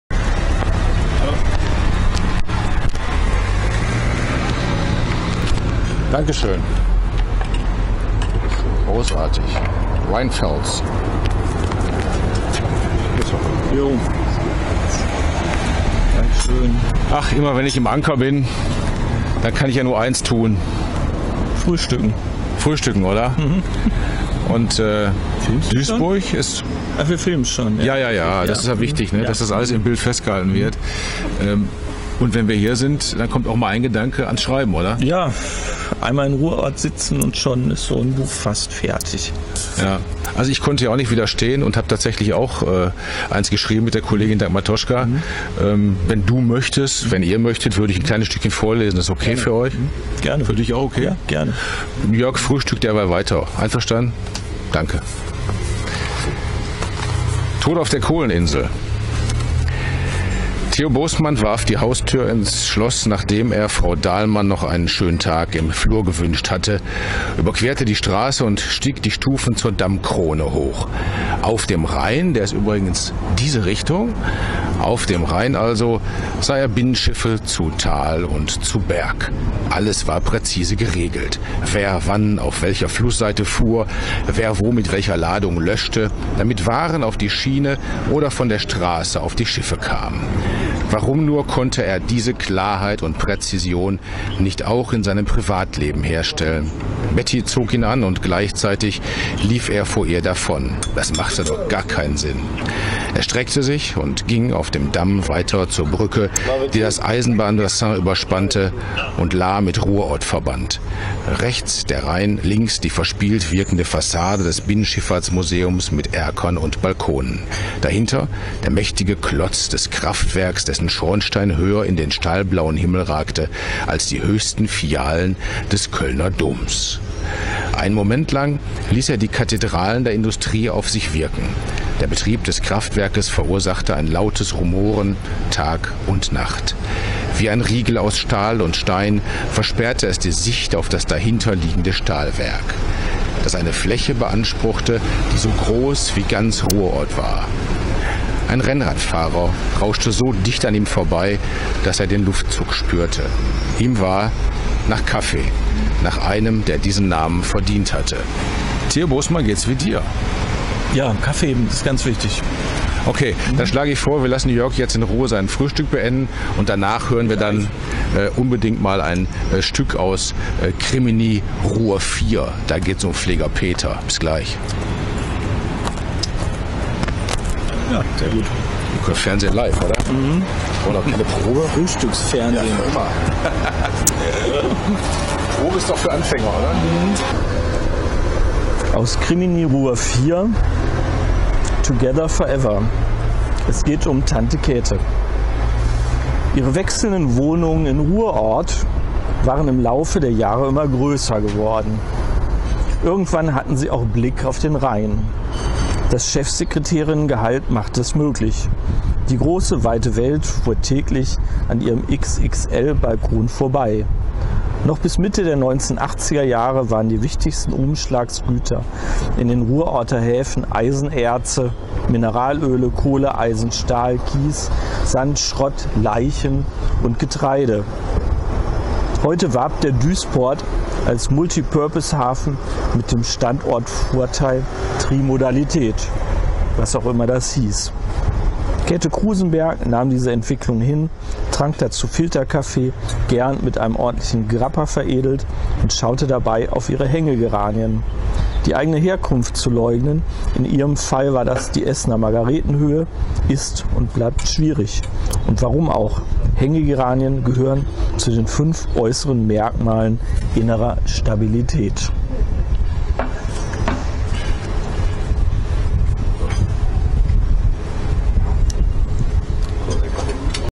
Kriminalromane, die in Duisburg-Ruhrort spielen
Ruhrort inspiriert Schriftsteller. Und macht hungrig. Während Arnd Rüskamp aus "Tod auf der Kohleninsel" liest, stärkt sich sein KRIMMINI-Compagnon Jörg Stanko erst einmal. Der trägt anschließend "Together forever" aus KRIMMINI RUHR 4 vor. Was hat das jetzt alles mit Horst Schimanski zu tun? Location: "Zum Anker". Infos